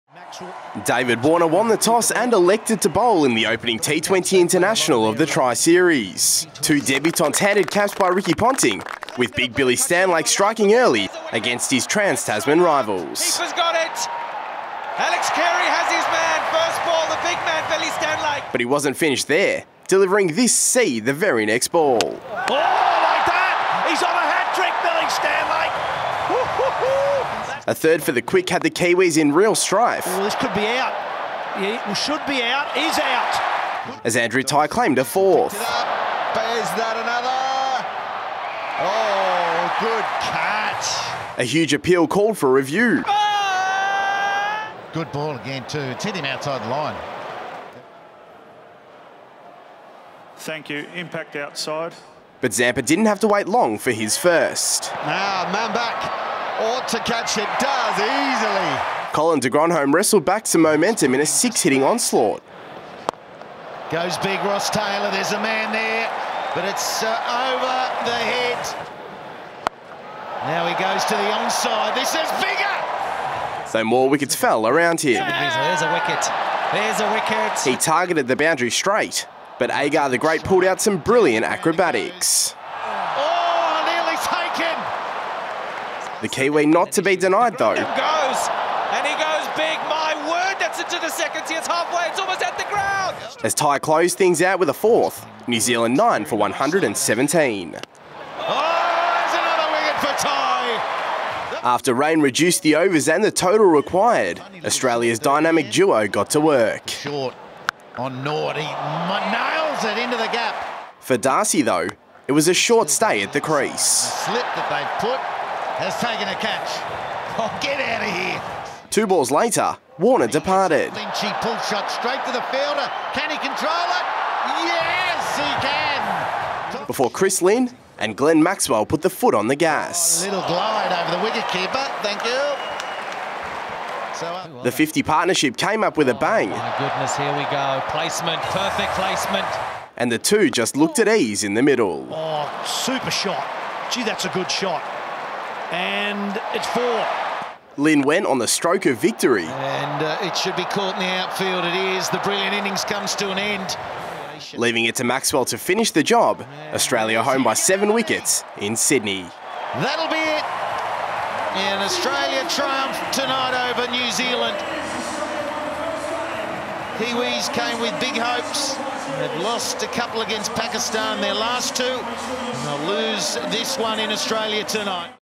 First T20: Australia v New Zealand
Billy Stanlake got the Aussies rolling, and Andrew Tye mopped up the tail, leaving Chris Lynn and Glenn Maxwell to chase down the required runs for a comprehensive T20 victory